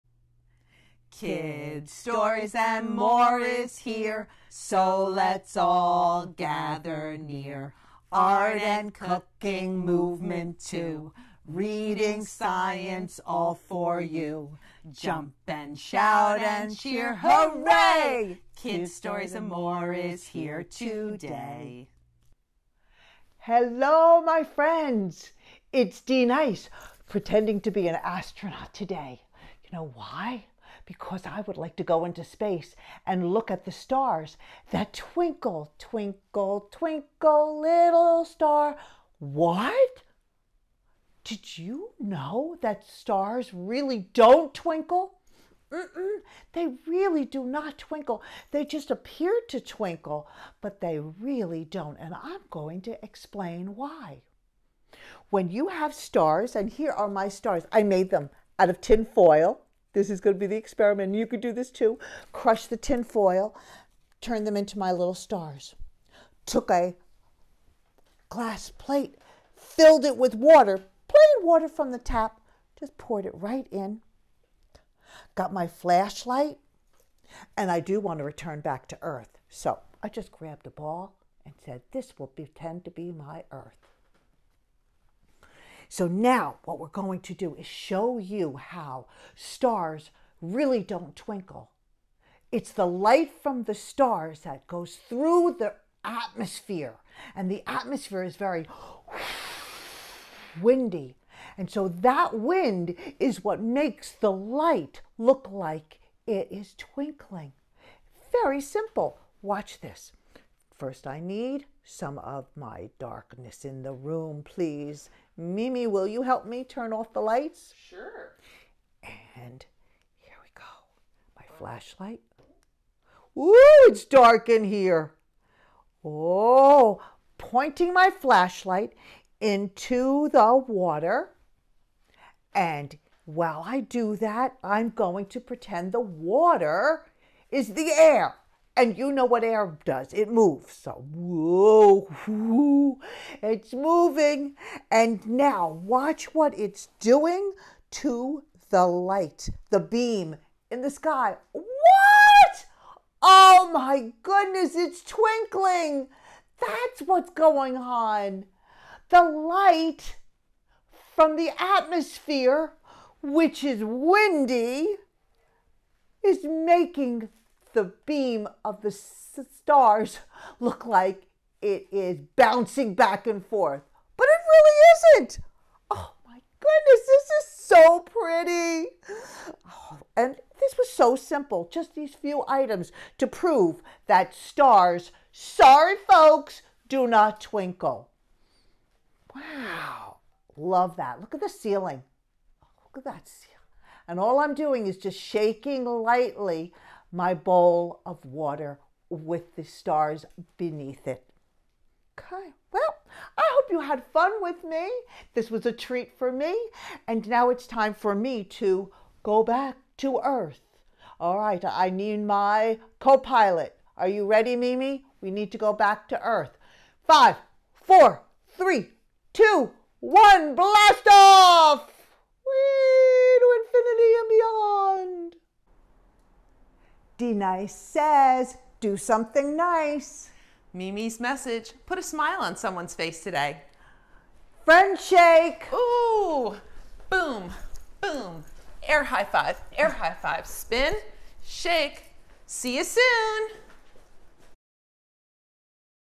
Kid's Science: Do Stars Really Twinkle? (Episode 13)
Enjoy this fun science experiment to teach this concept:
as light through a star races through our atmosphere, it bounces and bumps through the different layers, bending the light before you see it. Since the hot and cold layers of air keep moving, the bending of the light changes too, which causes the star's appearance to wobble or twinkle.